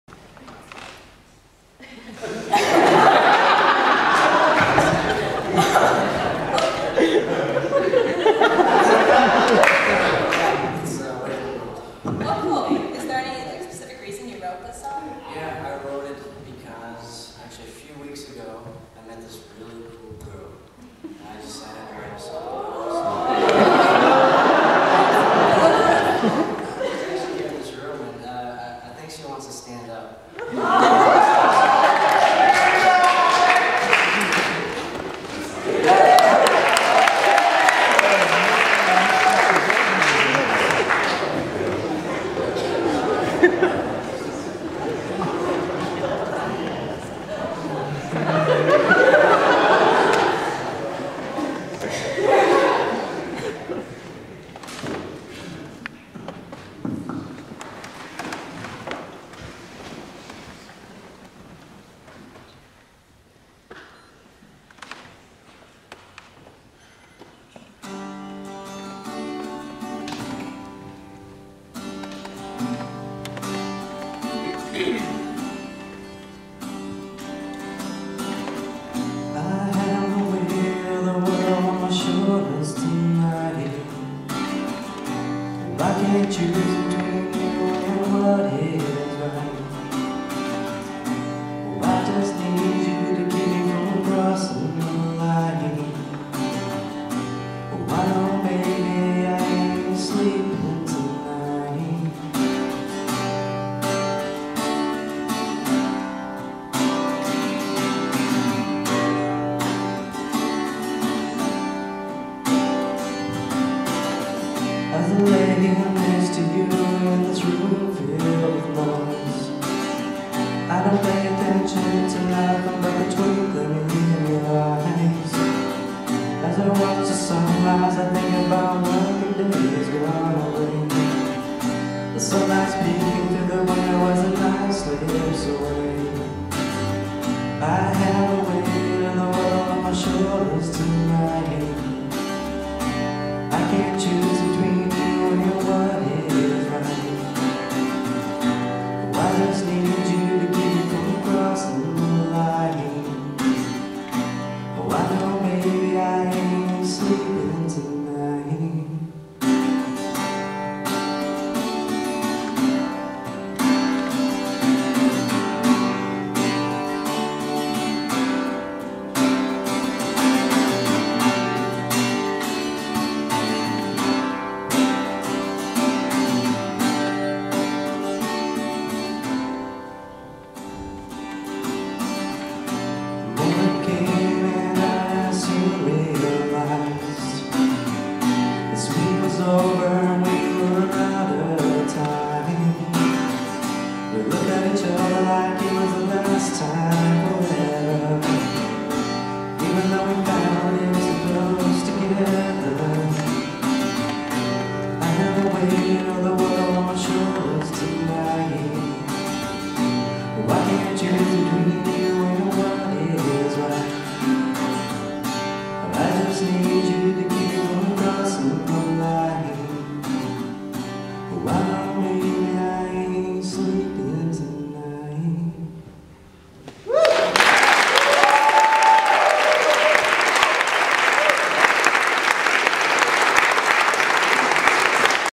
gage-talent-show-2011-song2.MPG
Gage's second song of the evening.  He just wrote this the week before the show;; he was still working on it the day of the show. It is inspired by his girlfriend as you will see at the beginning of the video.